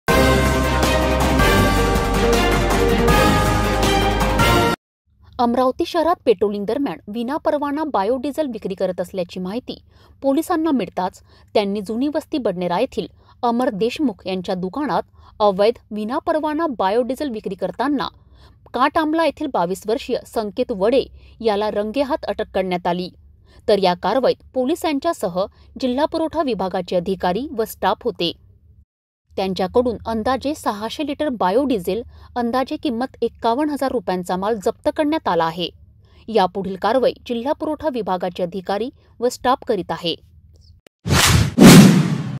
विनापरवाना बायोडिझेल विक्री वर गुन्हे शाखेकडून कारवाई..
अमरावती शहरात पेट्रोलिंग दरम्यान विनापरवाना बायोडिझेल विक्री करत असल्याची माहिती पोलिसांना मिळताच त्यांनी स्थानिक महानगरपालिका मार्केट नं. 7 , कोंडेश्वर रोड, जुनी वस्ती बडनेरा, अमरावती येथील अमर देशमुख यांचे दुकानात अवैध विनापरवाना बायोडिझेल विक्री करतांना आरोपी संकेत वडे 22, रा. काट आमला याला रंगे हात अटक करण्यात आली तर या कारवाईत पोलीस यांच्या सह जिल्हा पुरवठा विभागाचे अधिकारी व स्टाफ होते. त्यांचेकडून अंदाजे 600 लिटर बायोडिझेल ची अंदाजे किंमत 51 हजार रुपयांचा माल जप्त करण्यात आला आहे. यापुढील कारवाई जिल्हा पुरवठा विभागाचे अधिकारी व स्टॉफ करत आहेत.